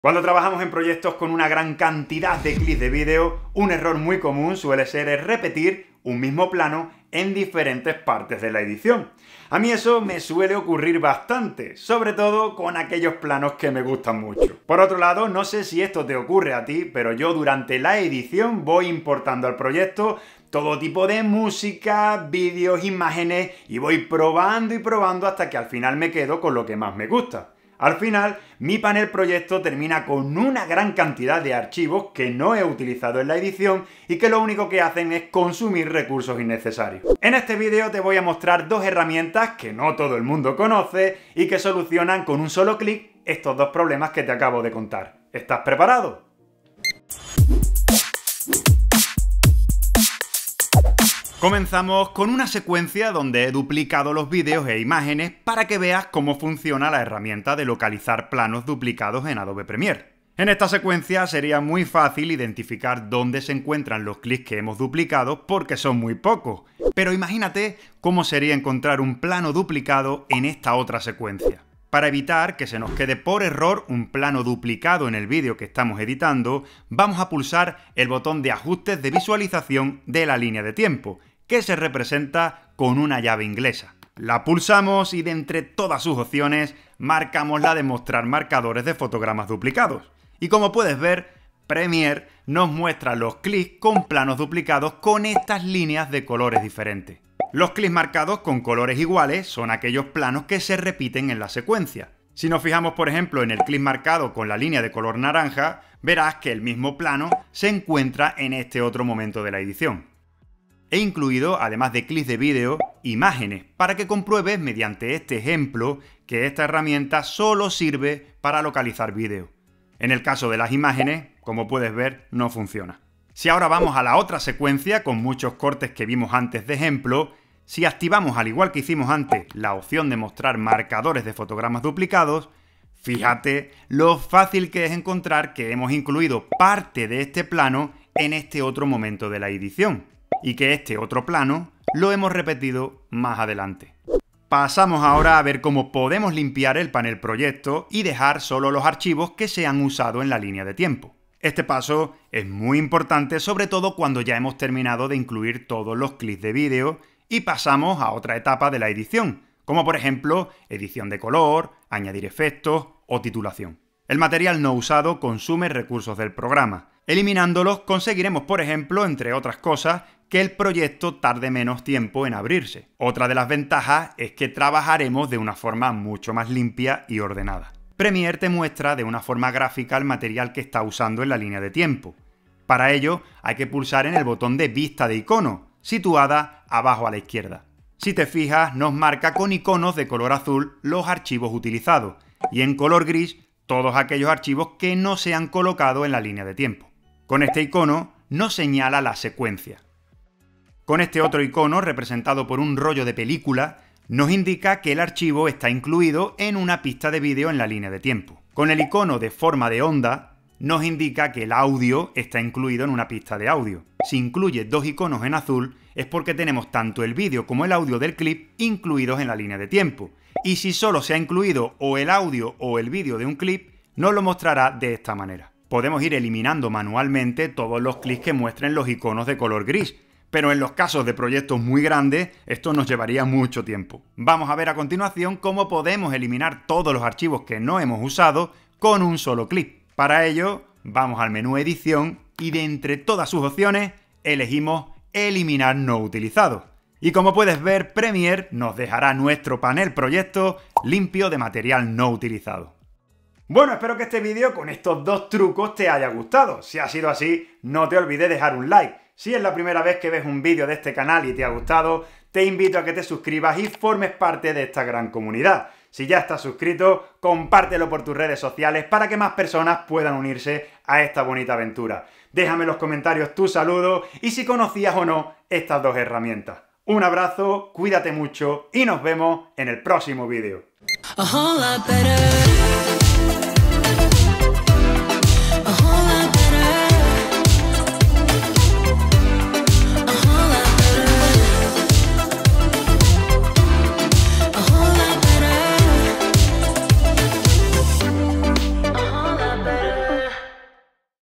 ¿Conoces estos 2 TRUCOS RÁPIDOS? - Premiere Pro
En este vídeo te voy a mostrar dos trucos rápidos que suelo usar en mis ediciones, por un lado cómo eliminar los archivos que no has usado en la edición y que consumen recursos del programa y por otro, cómo detectar si por error has usado un mismo plano varias veces en la línea de tiempo.

00:00 -  Intro - Tips para Editores
00:50 -  Mostrar marcadores de fotogramas duplicados en Adobe Premiere
02:24 - Eliminar archivos no utilizados en Adobe Premiere
04:13 - Despedida

★ EDITALO PRO ★